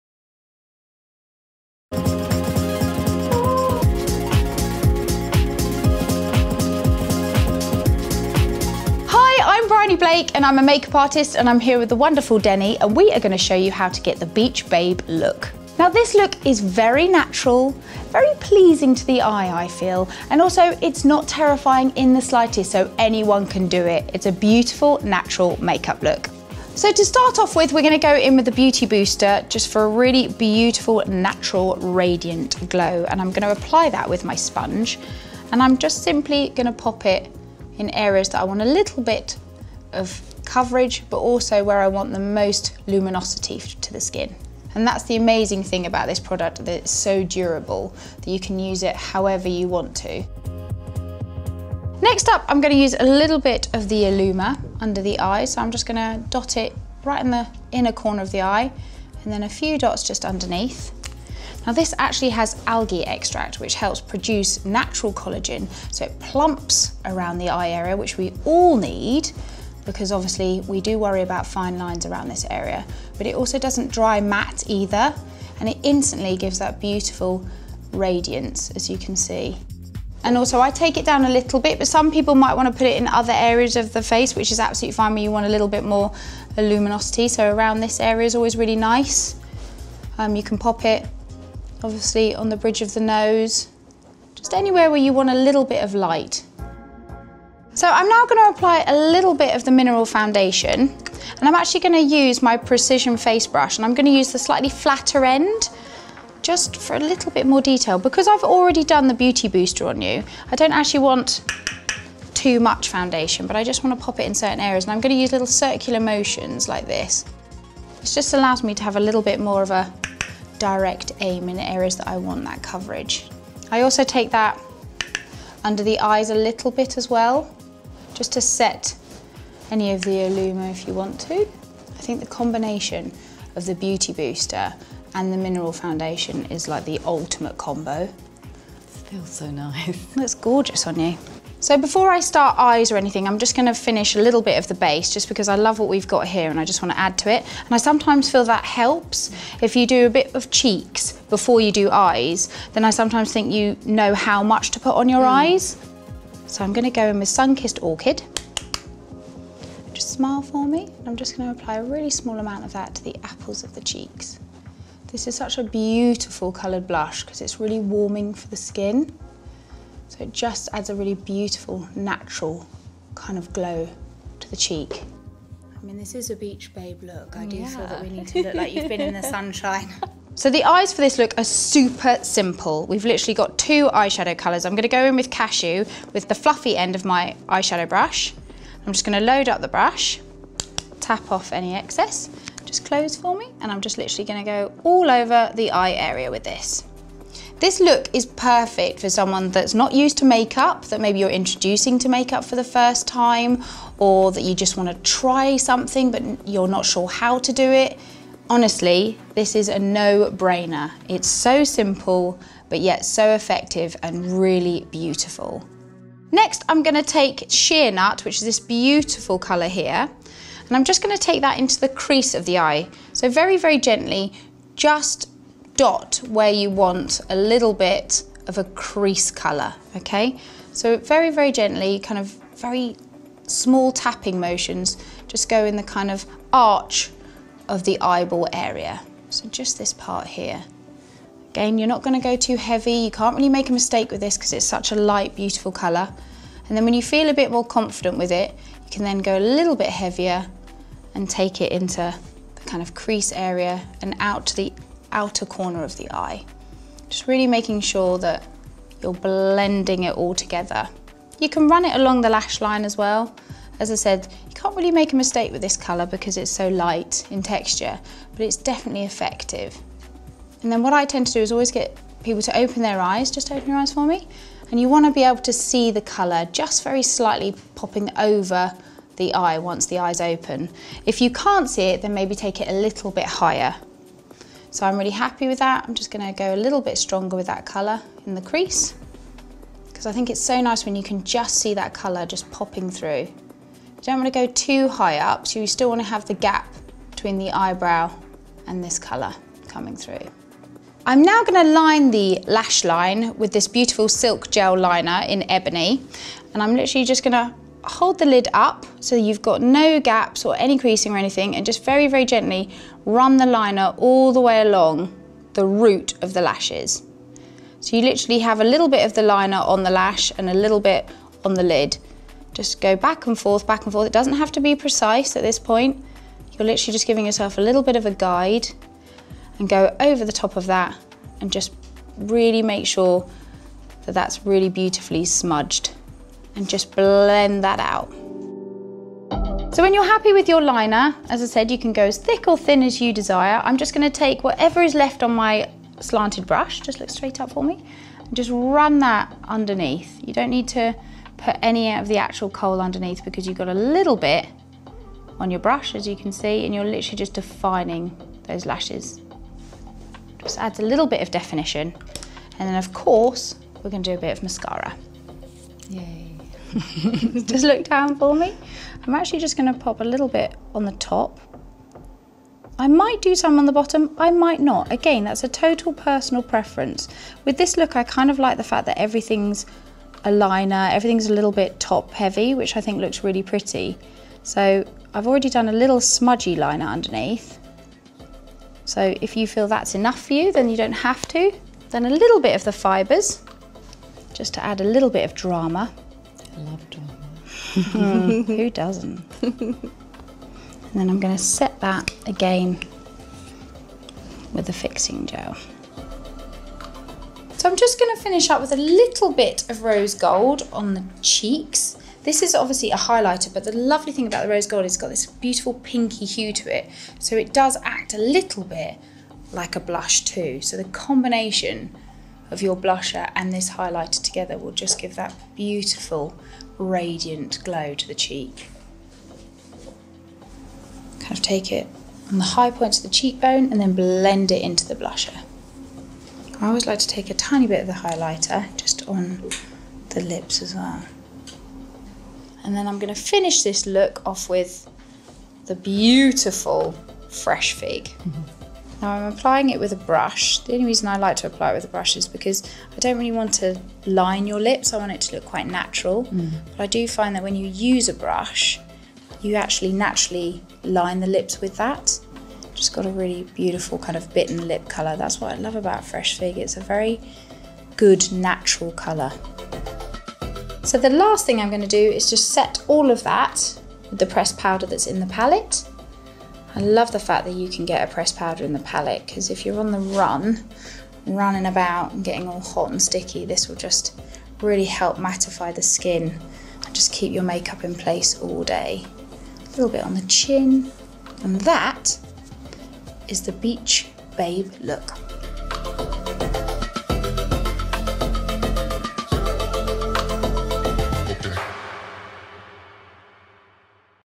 Simple, quick makeup demo
If, like me, makeup can feel a bit daunting and you just want someone to tell you exactly what to go for (absolutely me 🙋🏼‍♀️) then you’ll love this quick makeup demo.

Bryony Blake is a Makeup Artist who happens to love Tropic 🥰 She does makeup work for shows like X-Factor, Britain’s Got Talent & Dancing on Ice but what I really like about Bryony is that she makes makeup really simple and fun!

In this video she gives Déne the ‘Beach Babe’ look which is fresh, natural and would pretty much suit anyone!

These are the products that Bryony uses in the demo:

Beauty Booster - warm beige
Illuma concealer - latte
Mineral Foundation - warm beige

Colour Palette:

Cashew (s)
Shea Nut (s)
Sunkissed Orchid (m)
Rose Gold (m)
Make it Matt setting powder (l)

Silk Gel Liner - ebony

Fixing Gel Mascara (black)
Extension Fibres

Kiss Me Quick lipstick - Fresh Fig